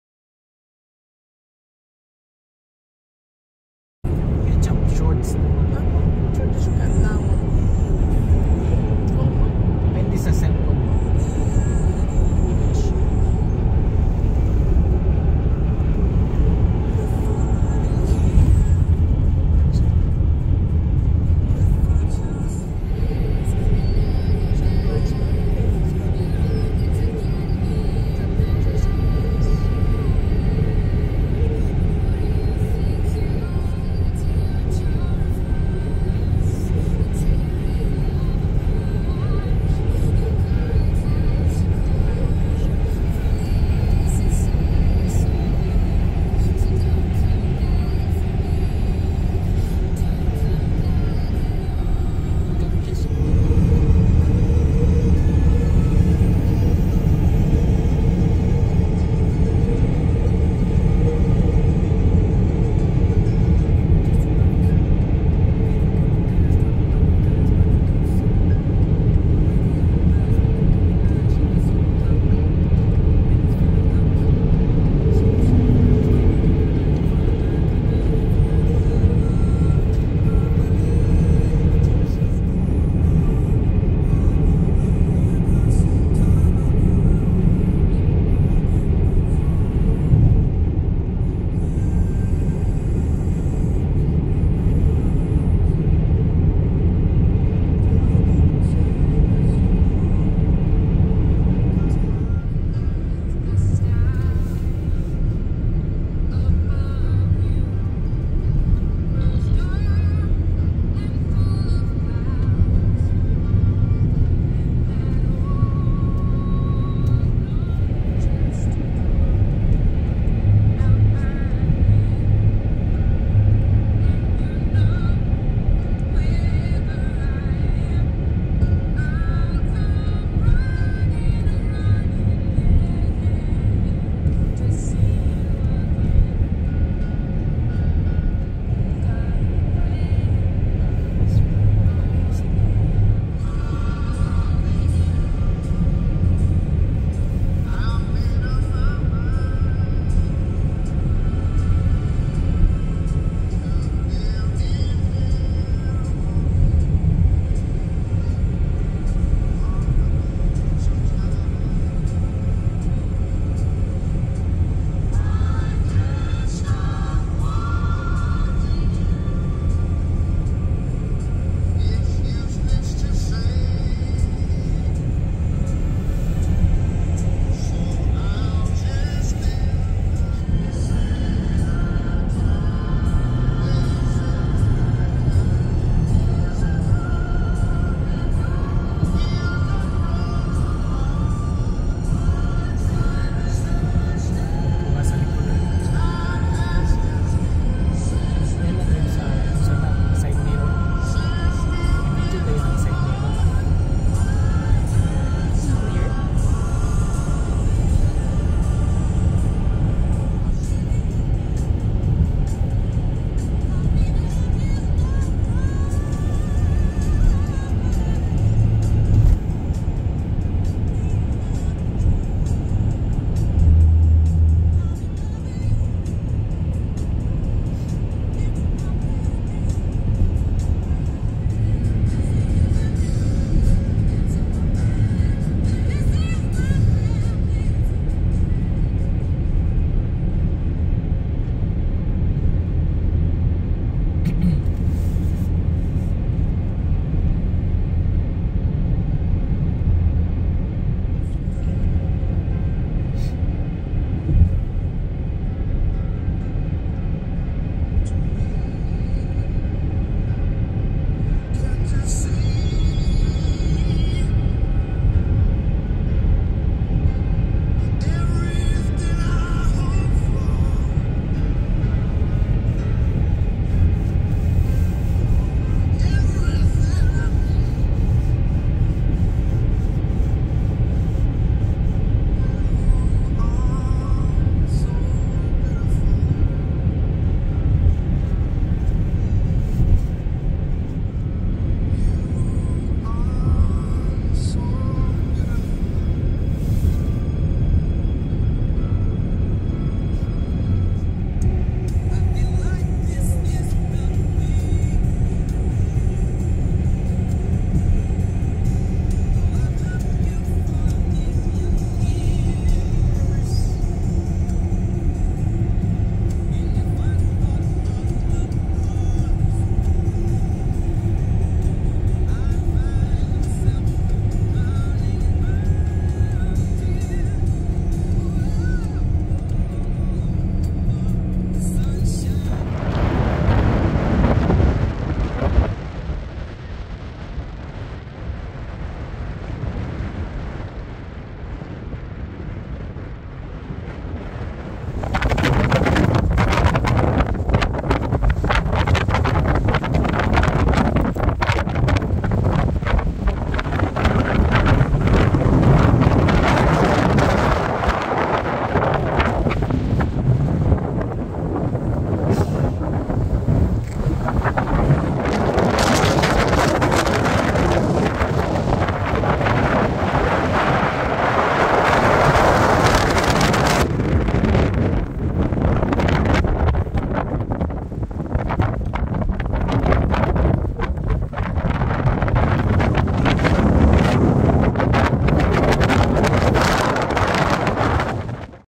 Calgary to Abraham Lake (Alberta, Canada) January 2023
One of those weekends when we spend time admiring the beauty of nature. A three and a half hour-drive from Calgary to Abraham Lake. Enjoy the scenic route ..as we did